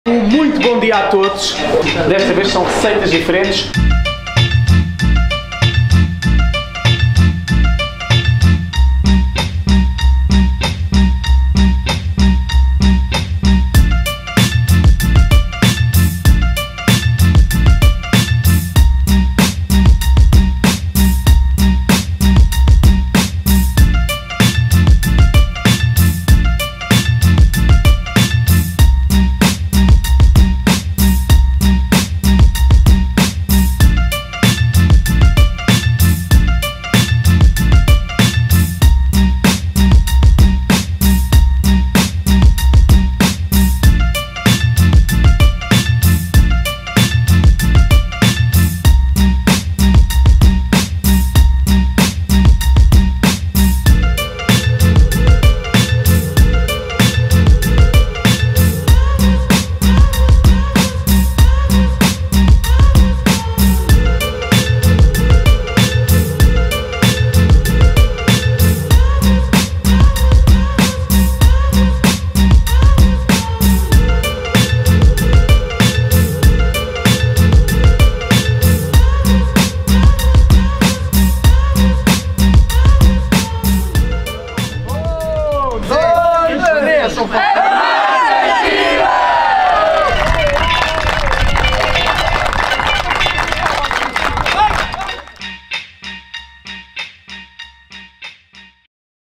O Chef Fábio Bernardino veio à aromáticas!
Mas que dia em grande. 😁
Na semana passada recebemos o nosso @chef_fabiobernardino para um dia bem recheado.
Cozinhamos, aprendemos, rimos e deliciamo-nos com os belos pratos preparados em conjunto.
Sem dúvida a repetir esta que já se tornou numa tradição Aromáticas Vivas. 💚🌿